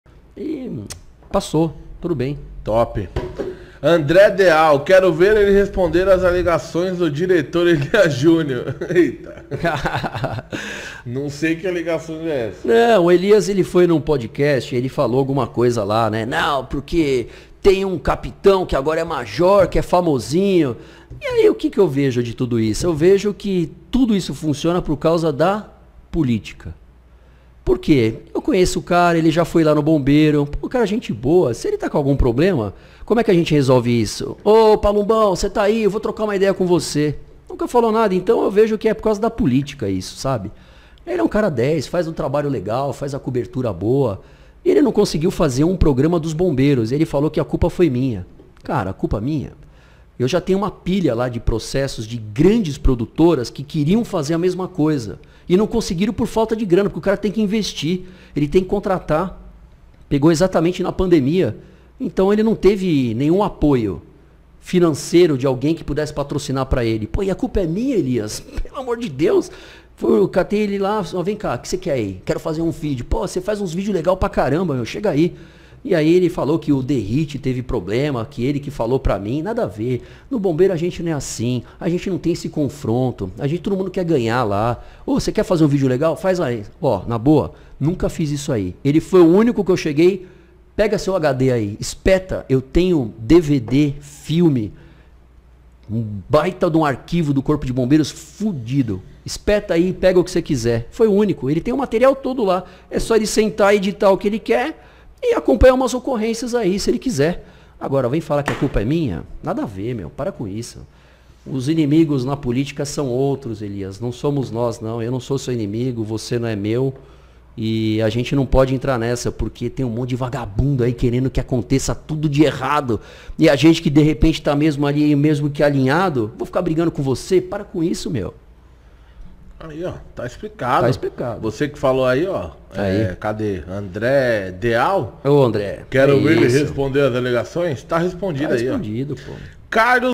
MAJOR PALUMBO COMENTA TRETA COM O ELIAS JUNIOR
Convidado : MAJOR PALUMBO